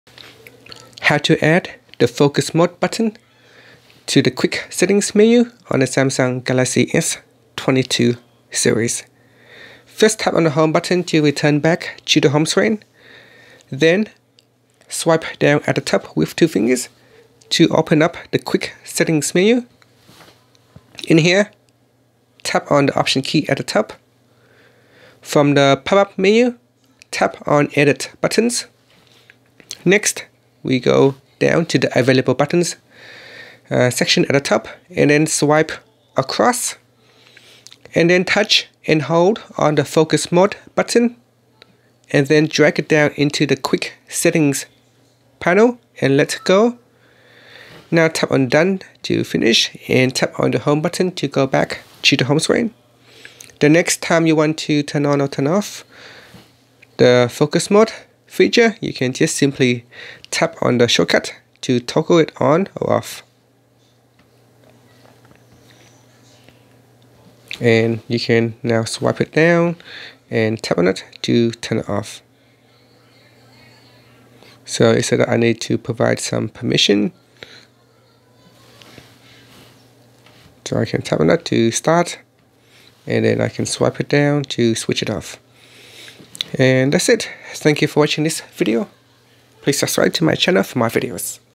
Galaxy S22/S22+/Ultra: How to Add Focus Mode Button to Quick Settings Menu
Learn how you can add focus mode button to quick settings menu on the Samsung Galaxy S22 / S22+ / S22 Ultra.